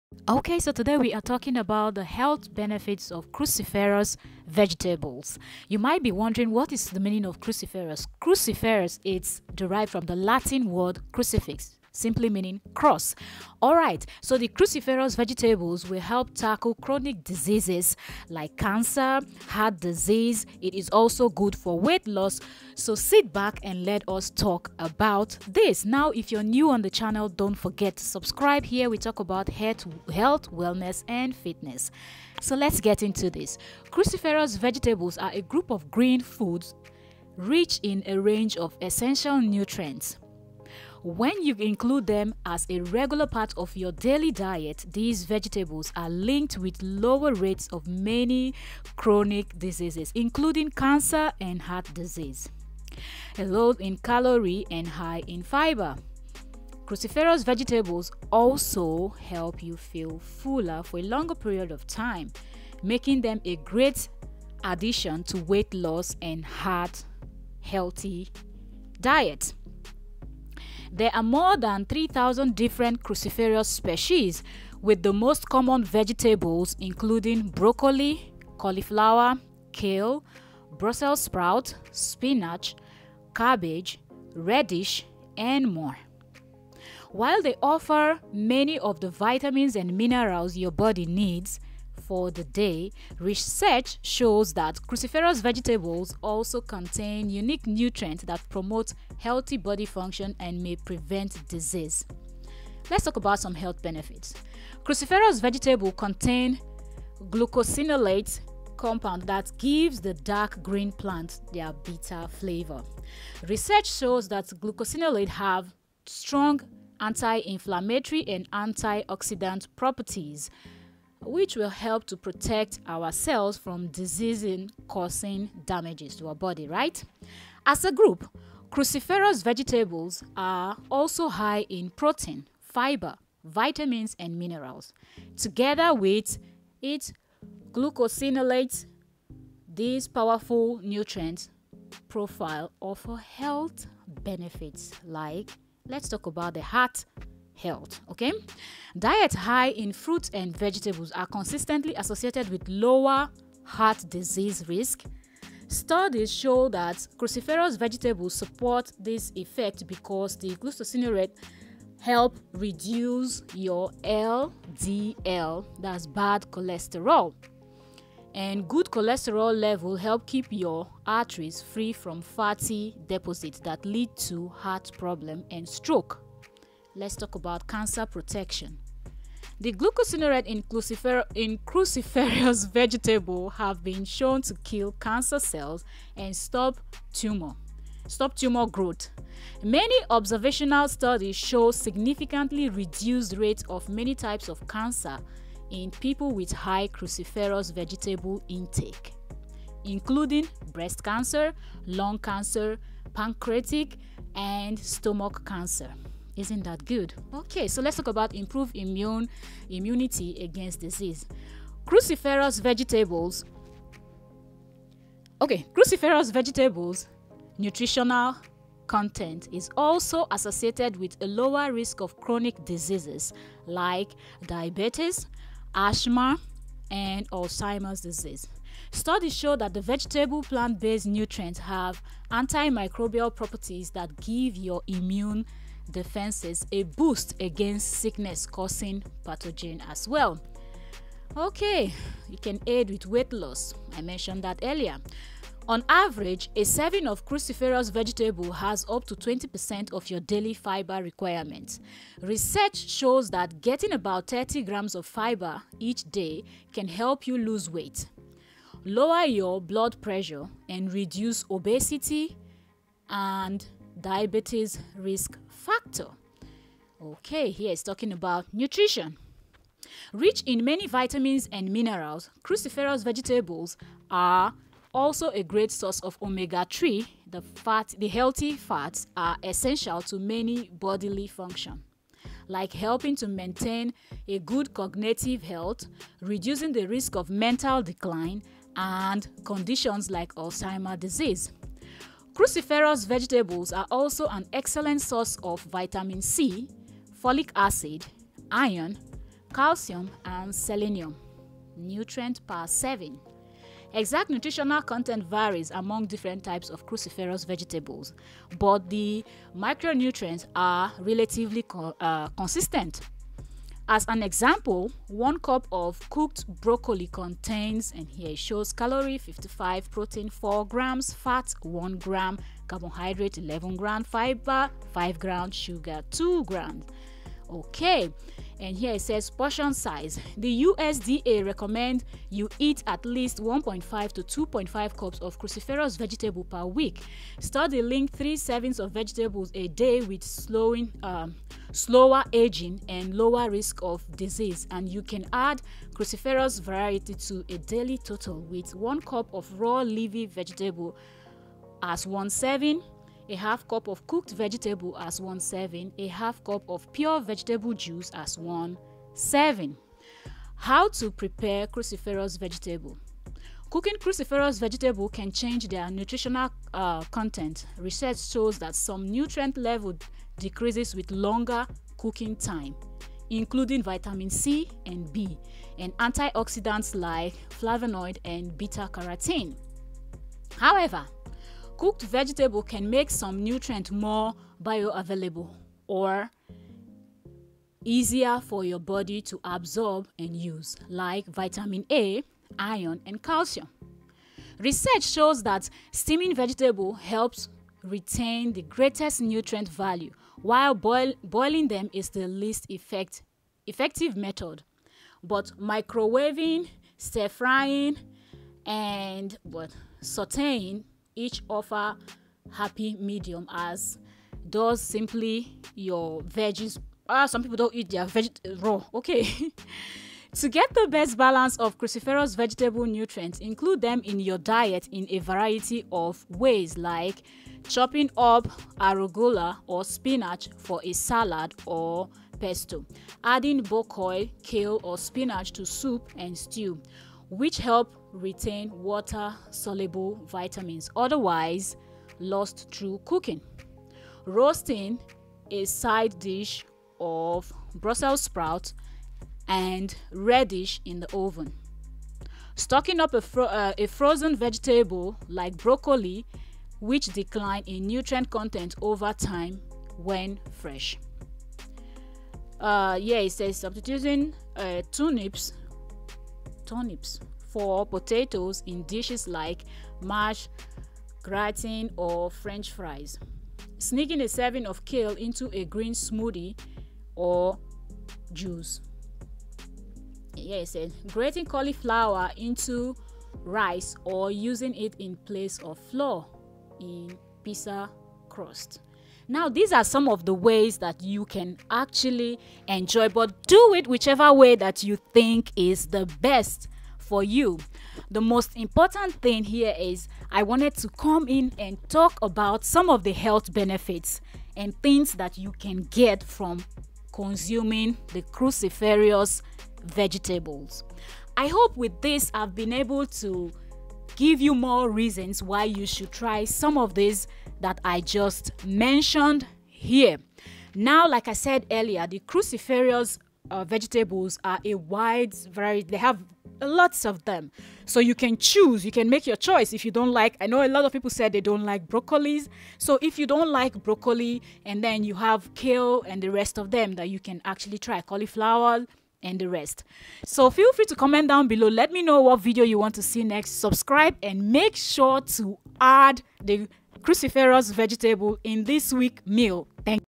Cruciferous Vegetables Health Benefits + How They Fight Chronic Diseases
On today's health and wellness talk we will be looking at the various health benefits of the cruciferous vegetables.